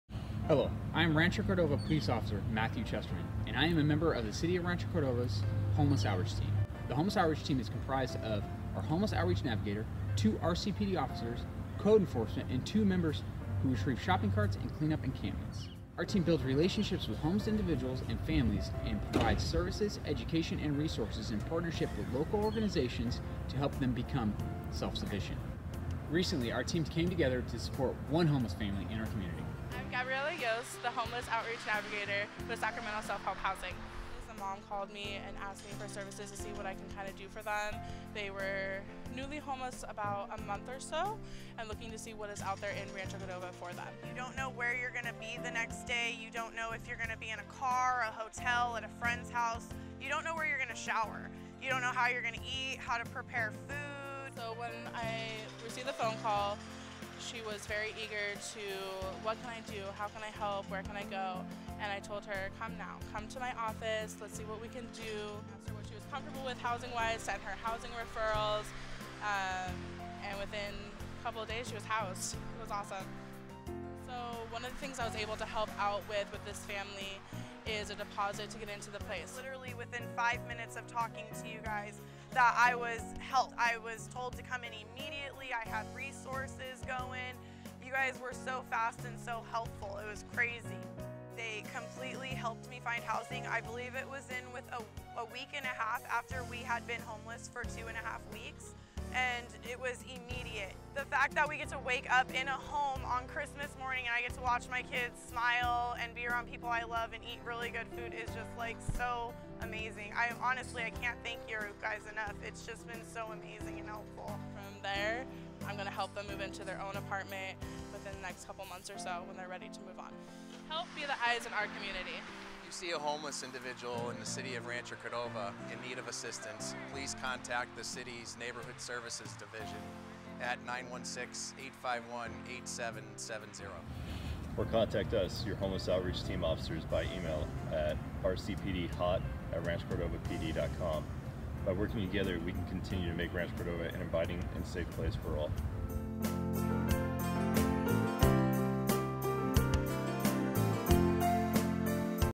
A Home for the Holidays
A Rancho Cordova family of four was experiencing homelessness in our community. The family learned about the City of Rancho Cordova’s Homeless Outreach Team, which is comprised of our Homeless Outreach Navigator, two Rancho Cordova Police Department Officers, code enforcement and two members who retrieve shopping carts and clean up encampments, and reached out for assistance with finding housing and other services. Watch this video to learn how the family went from homelessness to having a home for the holidays.